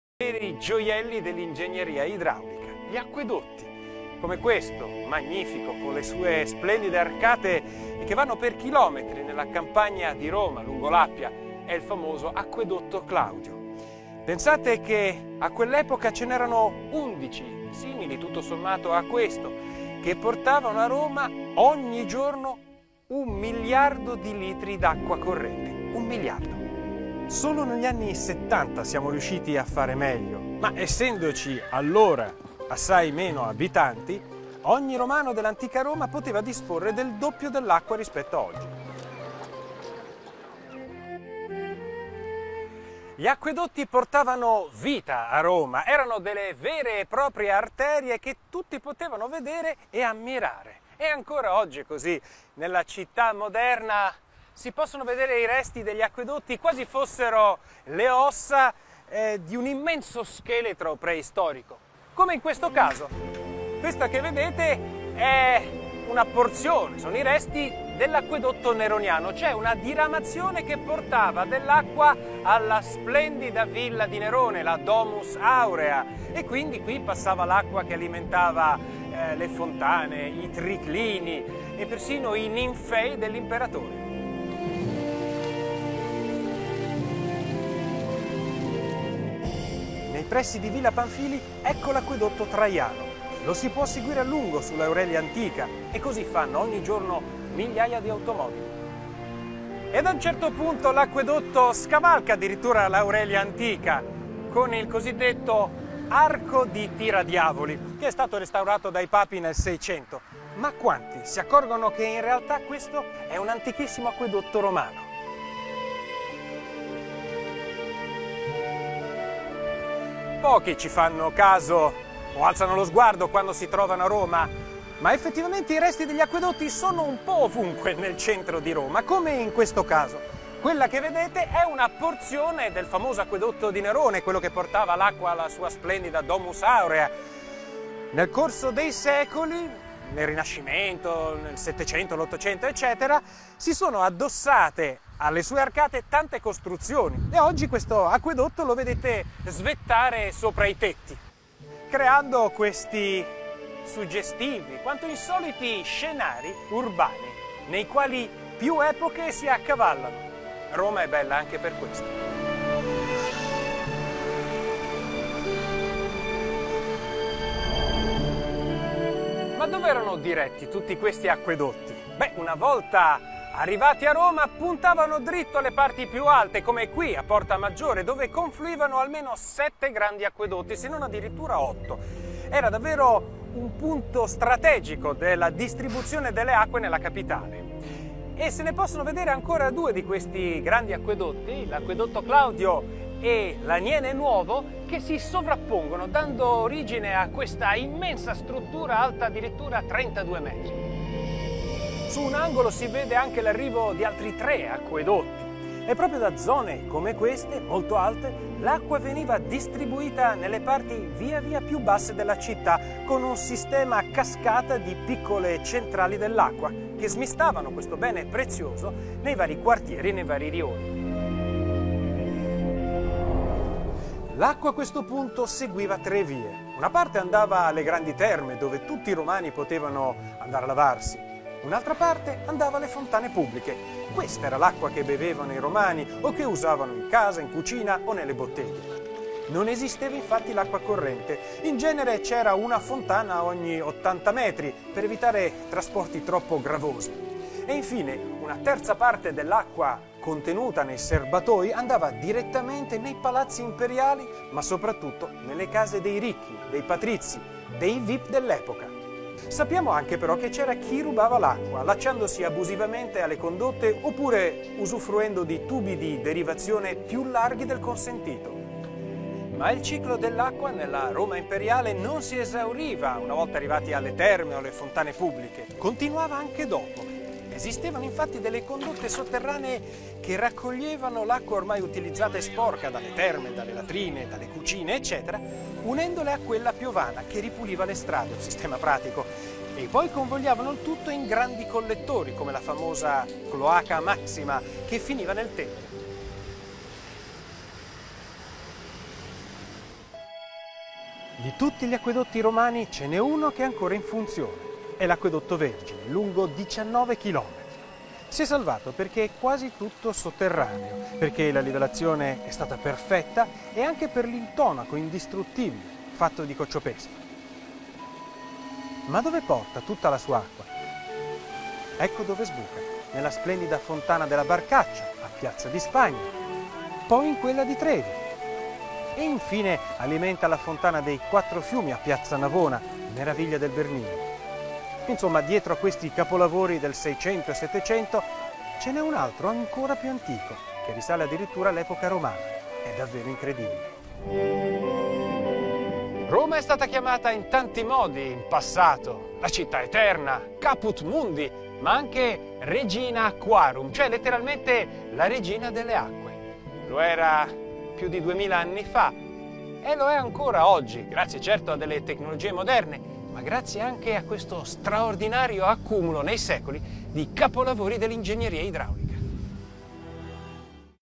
regina aquarum
Sai che a Roma... la città era chiamata anche Regina Aquarum?
Ecco perché!